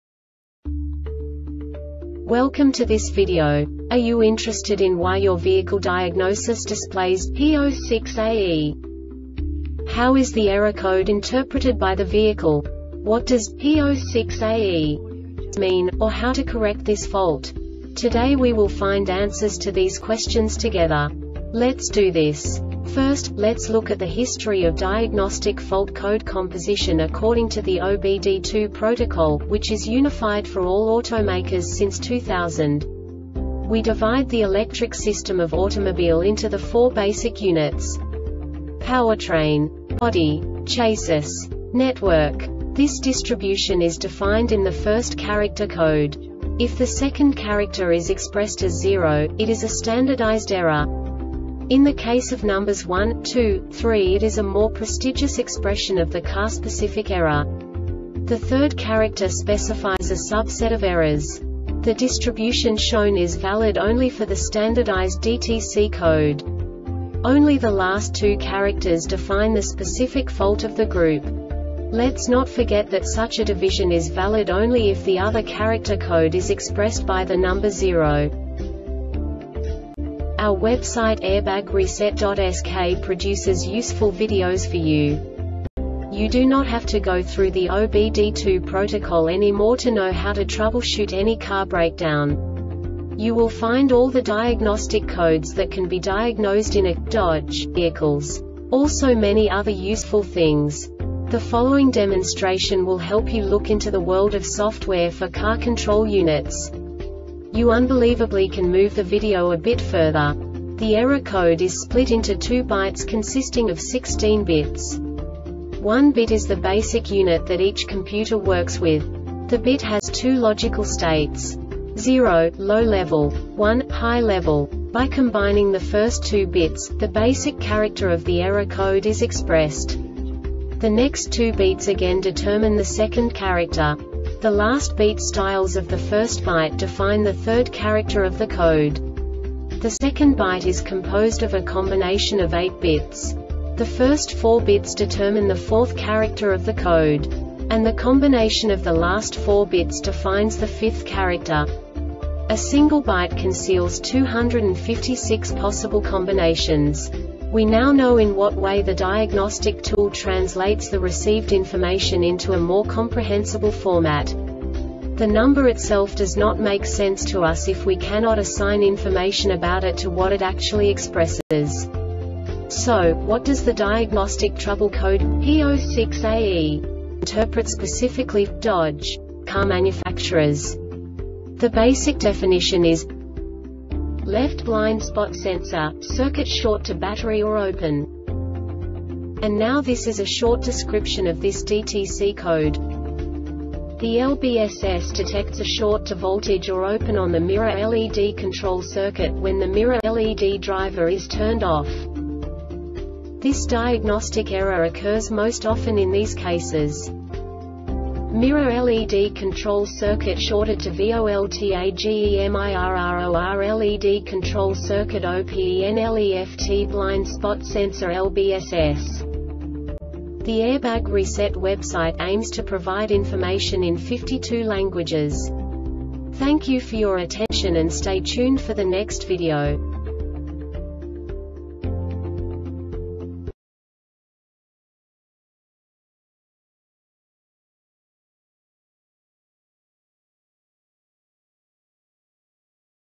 DTC Dodge P06AE Short Explanation
Contents:
0:21 Basic DTC analysis according to OBD2 protocol standard.
1:48 Insight into programming
2:58 A diagnostically understandable description of the Diagnostic Trouble Code
3:05 Basic error definition

Make:
Dodge
Code:
P06AE
Definition:
Left Blind Spot Sensor - Circuit Short To Battery Or Open
Description:
The LBSS detects a short to voltage or open on the Mirror LED Control circuit when the Mirror LED Driver is turned off.
Cause:
MIRROR LED CONTROL CIRCUIT SHORTED TO VOLTAGEMIRROR LED CONTROL CIRCUIT OPENLEFT BLIND SPOT SENSOR (LBSS)